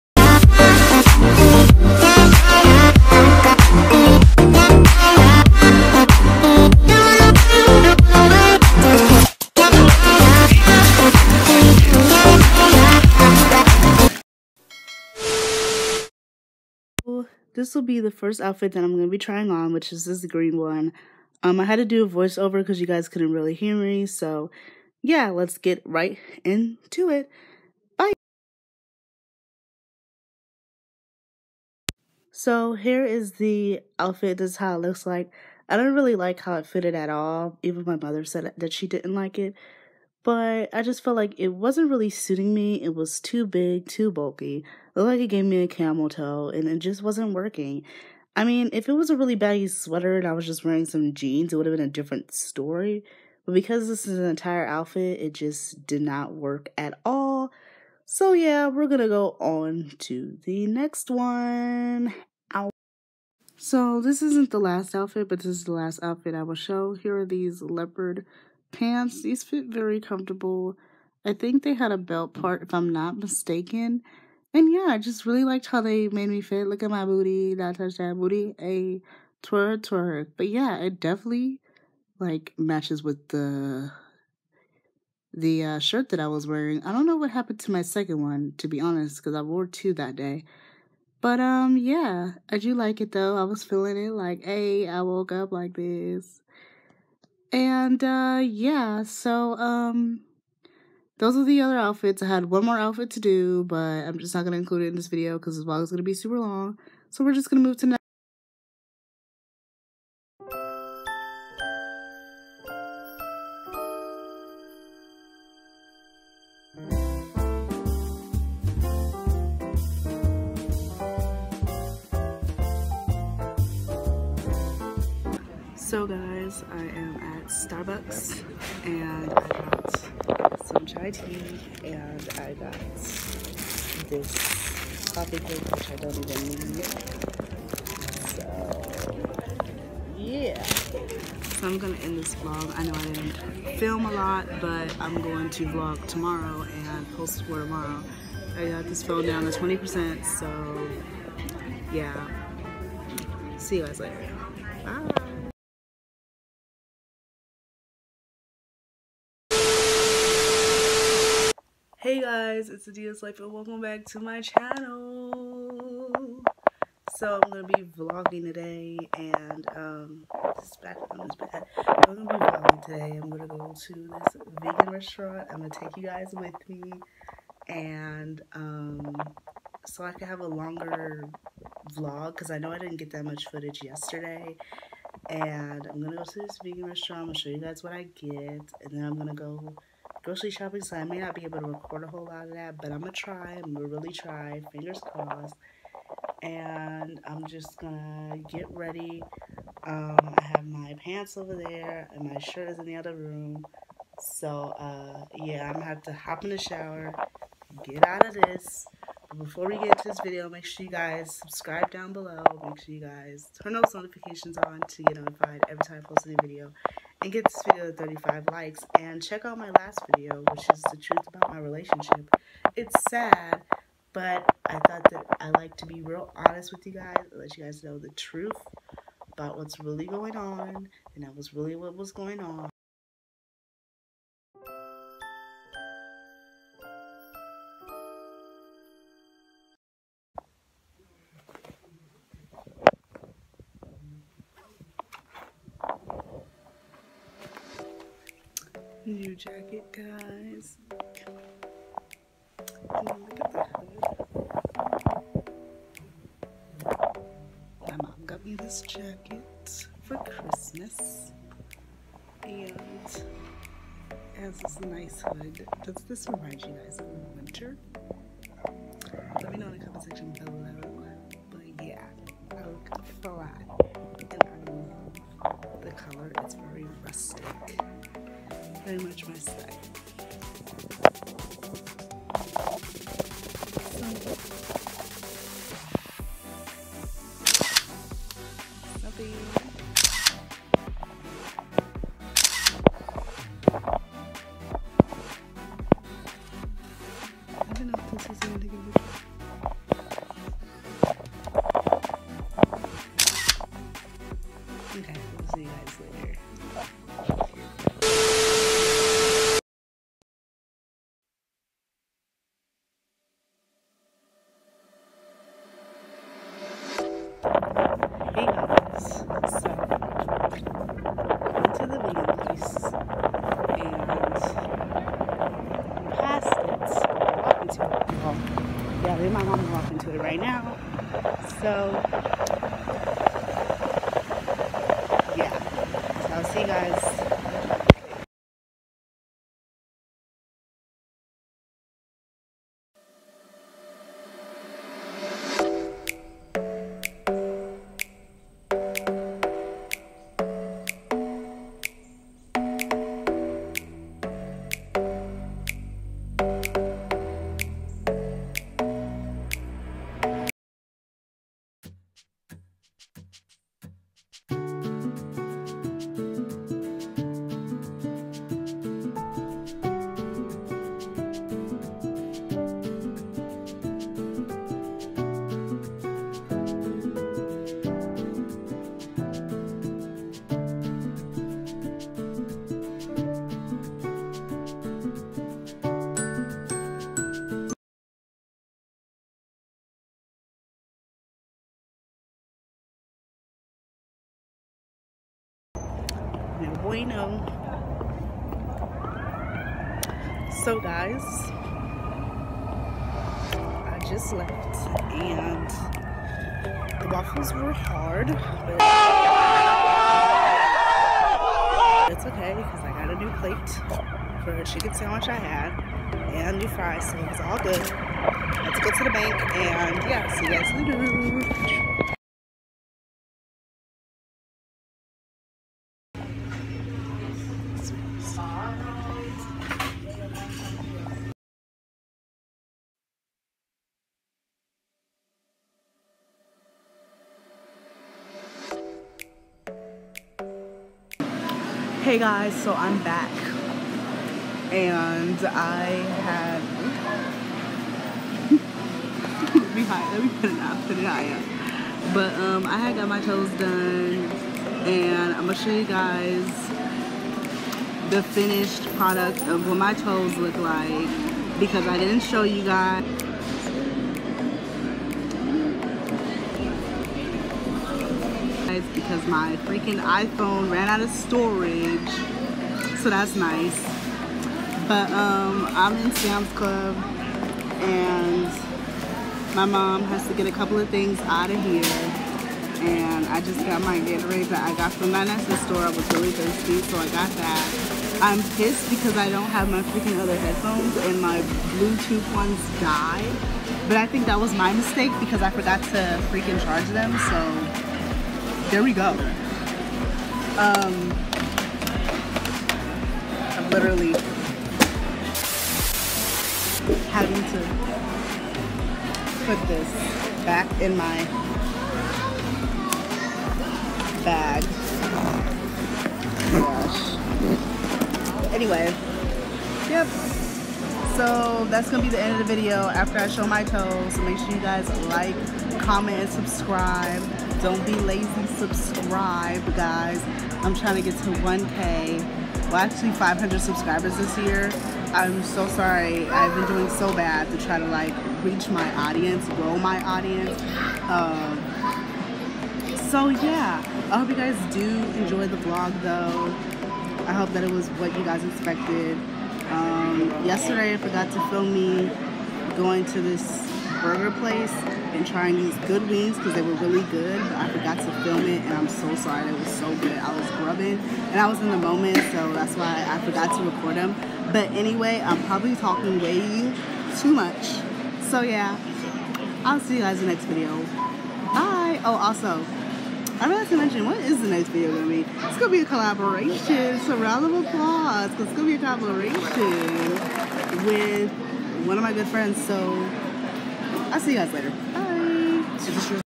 WEEKEND VLOG WITH MOM
So in this Vlog I went to target to look for outfits , Starbucks , get my toes done and had went to this vegan restaurant and ate some chicken & waffles but waffles sucked 😂.